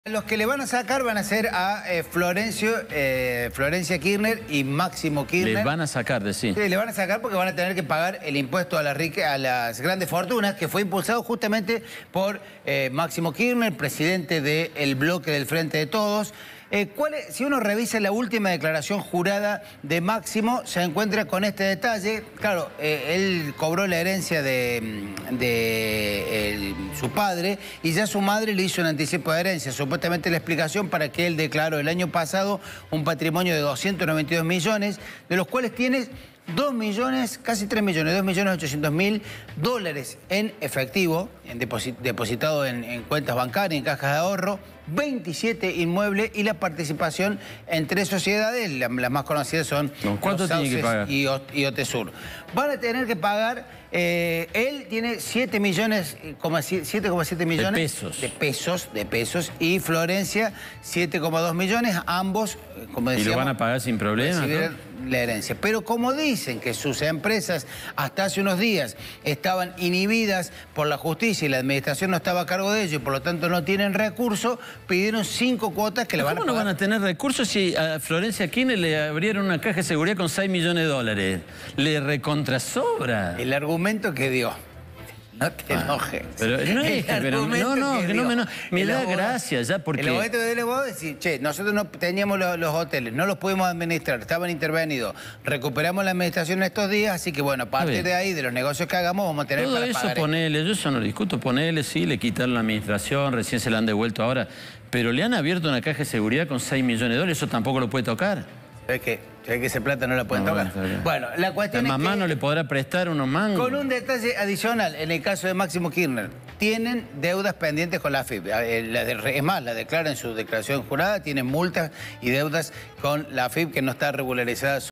Máximo y Florencia Kirchner pagarán el “impuesto a la riqueza” en cuotas
Máximo Kirchner pagará el “Impuesto a la riqueza”. Pero lo hará en cuotas. Así se lo pidió a la Administración Federal de Ingresos Públicos (Afip).
El diputado nacional, hijo de la vicepresidenta, deberá pagar 7,7 millones de pesos correspondiente al Aporte Solidario y Extraordinario. La solicitud de ingresar al plan de pagos alcanza también a su hermana, Florencia Kirchner, quien deberá abonar un monto similar.
Según una resolución judicial, del año pasado, los Kirchner podrán pagar los impuestos generados por el patrimonio intervenido con fondos que se encuentran bajo administración judicial. En este caso, según detalla María Cecilia García, la contadora de los Kirchner, la intervención judicial le informó que no tenía disponibles los fondos para pagar el 100% del impuesto promovido por el Gobierno nacional.